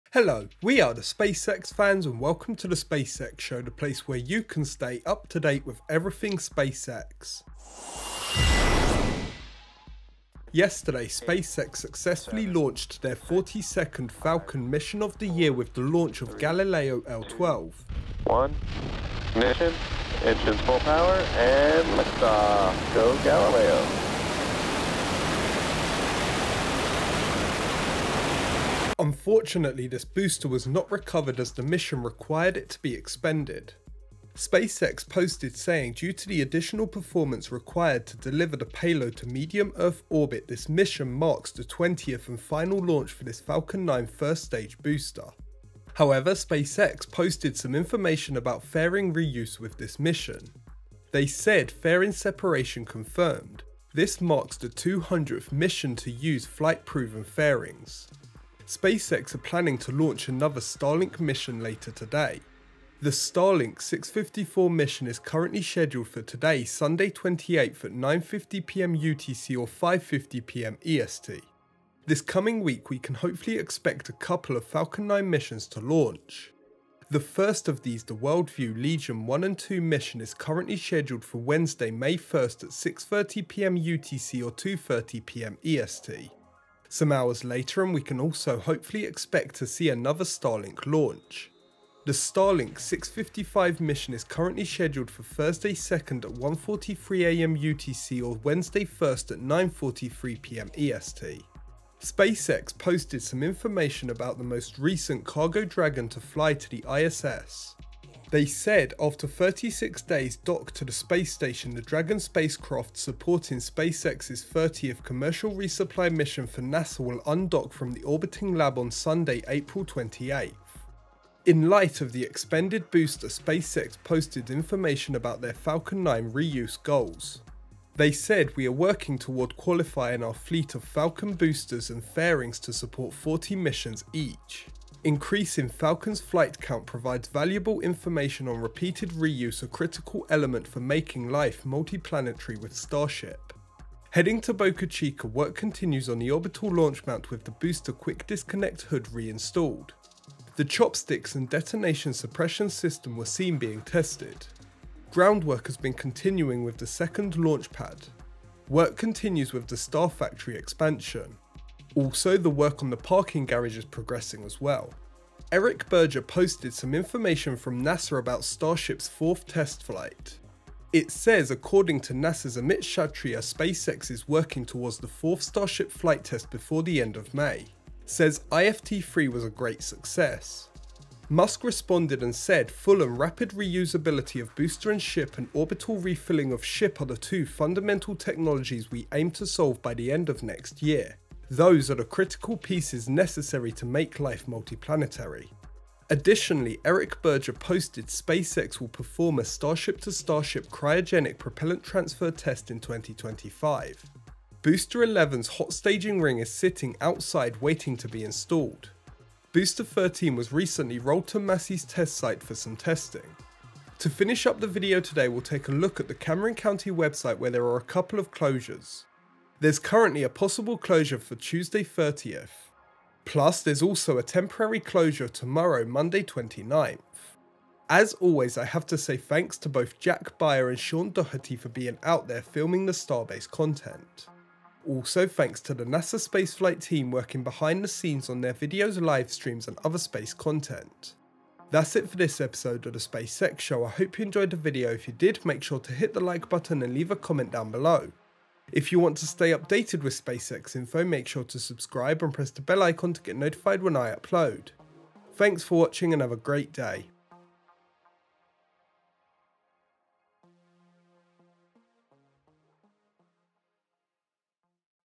SpaceX Starship Updates! Booster 13 Rolled Out For Testing at Starbase! TheSpaceXShow
Welcome to the latest SpaceX Starship updates video from TheSpaceXFans. We have a launch and other related updates for the standard SpaceX news. Over in Boca Chica, work continues on both infrastructure and prototypes as they prepare for Starship flight 4. Also, there’s been Booster 13 rolled out for testing at Starbase! Sit back and enjoy!

Sources / Accreditations:
SpaceX. Liftoff! X. SpaceX. April 28, 2024.

Jack Beyer & Sean Doherty & Starbase Live. Booster QD Cover Reinstalled, GSE Tested | SpaceX Boca Chica. YouTube. NASASpaceflight. April 27, 2024.